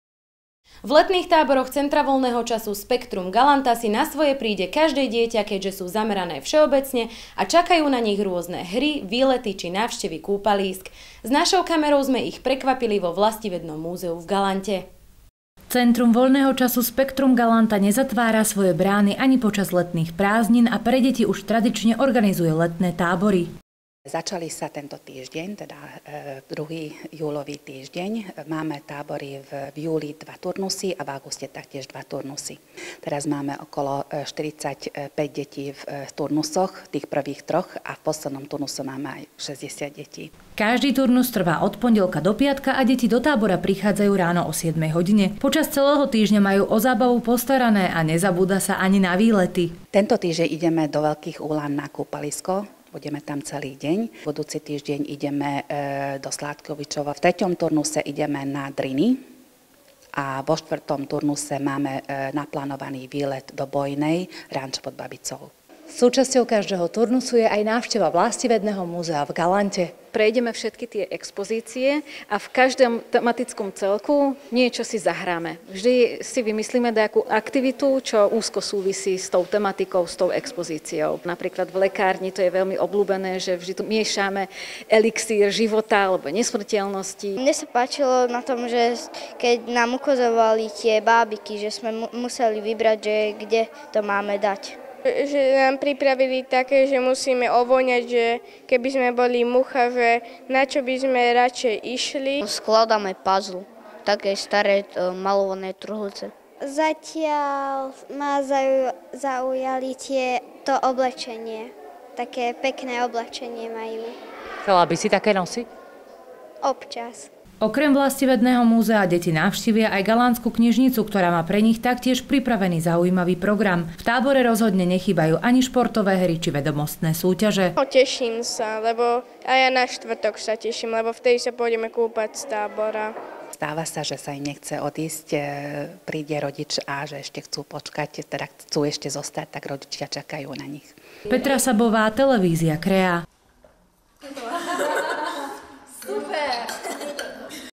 LETNÝ TÁBOR V CVČ GALANTA
V letných táboroch CVČ - Spektrum Galanta si na svoje príde každé dieťa, keďže sú zamerané všeobecne a čakajú na nich rôzne hry, výlety či návštevy kúpalísk. S našou kamerou sme ich prekvapili vo Vlastivednom múzeu v Galante